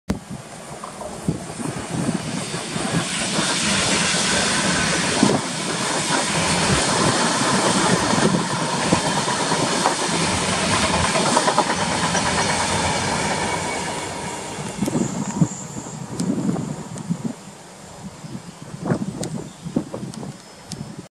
66736
66736 passing Thankerton with 6S45 the 0714 North Blyth to Fort William on Saturday 21 September 2019